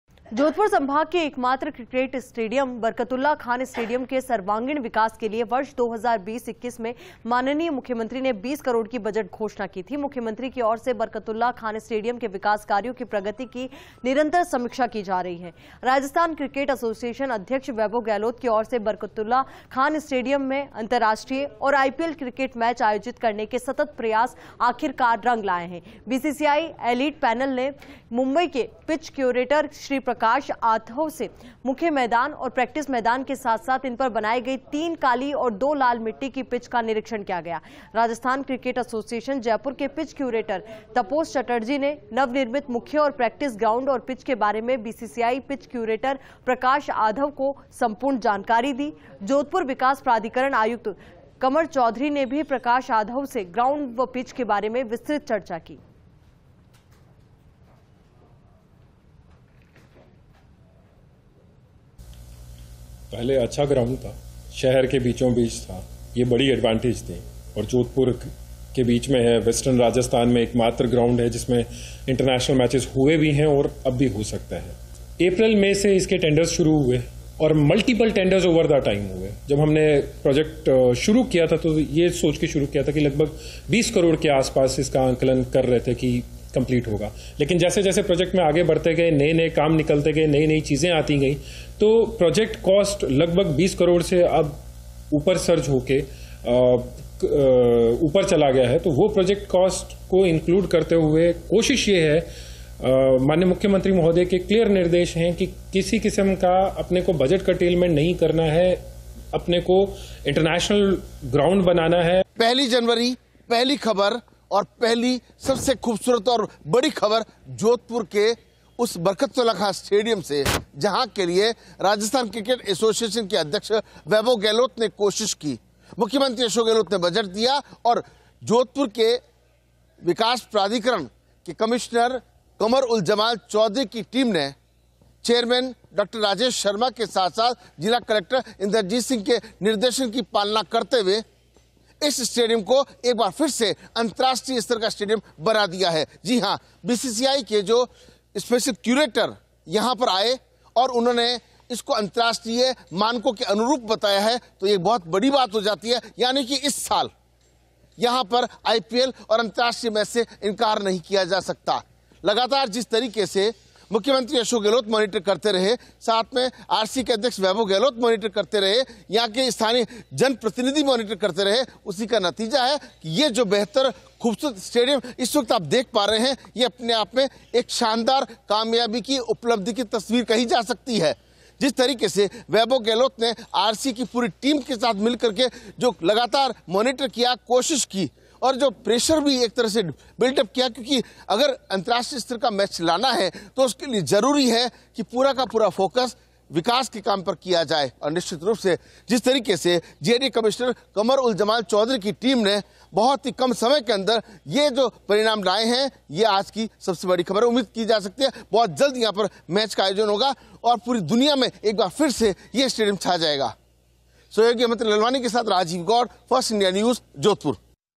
बरकतुल्लाह खान स्टेडियम में विकास कार्य जारी, BCCI की ओर से किया ग्राउंड का निरिक्षण । Jodhpur News
About this Video:
Development work continues at Barkatullah Khan Stadium, ground inspected by BCCI. Jodhpur News
About Channel: